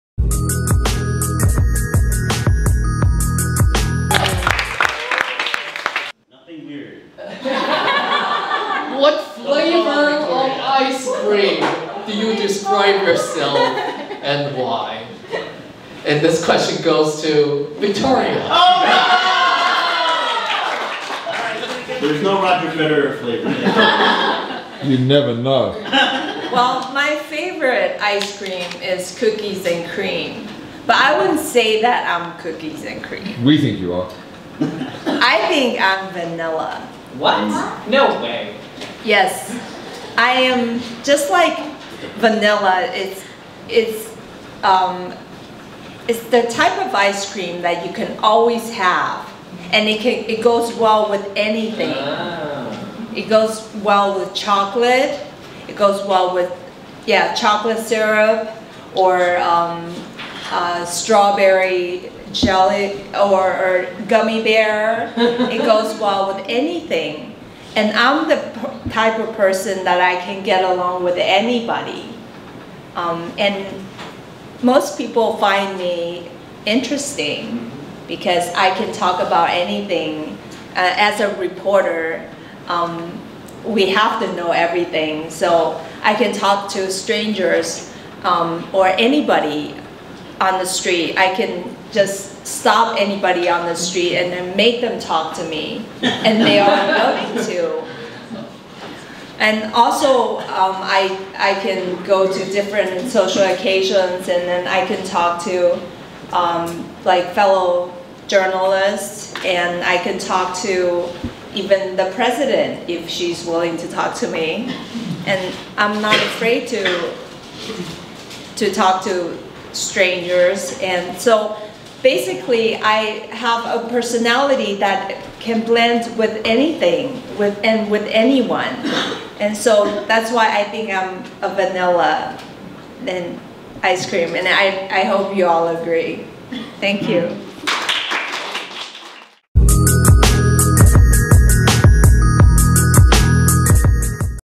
[What flavor of ice cream are you? Why? || Victoria Jen Table Topics]
Speaker: Victoria Jen
Host: Tim Huang
Word of the Day: Chill
TGIF Meeting No.:  275
Theme: Just Chill 消暑一夏
Date: 7  July 2023
Videographer:
More from Victoria Jen 任百珺